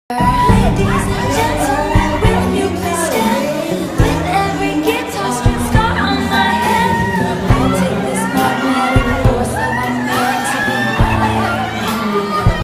RK Bagatsing and Jane Oineza at the SRRExtreme premiere
Real life couple RK. Bagatsing and Jane Oineza at the premiere night of Shake Rattle and Roll Extreme